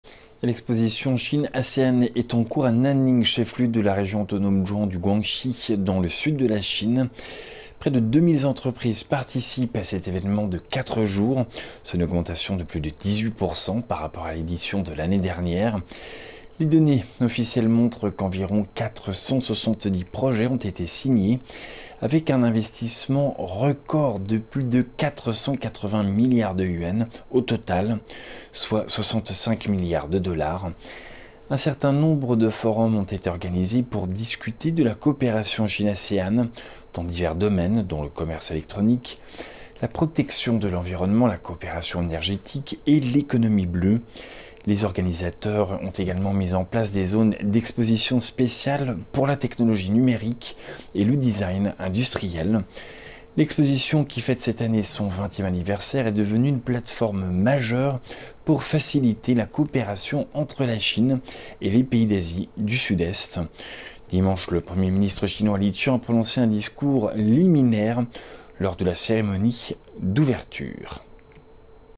Un record de 470 projets signés pour une valeur totale de plus de 480 milliards de yuans
Expo Chine-ASEAN : un record de 470 projets signés pour une valeur totale de plus de 480 milliards de yuans
L'Exposition Chine-ASEAN se tient présentement à Nanning, chef-lieu de la région autonome Zhuang du Guangxi, dans le sud de la Chine. Près de 2000 entreprises participent à cet événement de quatre jours, soit une augmentation de plus de 18 % par rapport à l'édition de l'année dernière. Les chiffres officiels montrent qu'environ 470 projets ont été signés, pour un investissement total record de plus de 480 milliards de yuans, soit 65 milliards de dollars. Un certain nombre de forums ont été organisés pour discuter de la coopération Chine-ASEAN dans divers domaines, dont le commerce électronique, la protection de l'environnement, la coopération énergétique et l'économie bleue. Les organisateurs ont également mis en place des zones d'exposition spéciales pour la technologie numérique et le design industriel. L'exposition, qui fête cette année son 20e anniversaire, est devenue une plateforme majeure pour faciliter la coopération entre la Chine et les pays d'Asie du Sud-Est. Dimanche, le Premier ministre chinois Li Qiang a prononcé un discours liminaire lors de la cérémonie d'ouverture.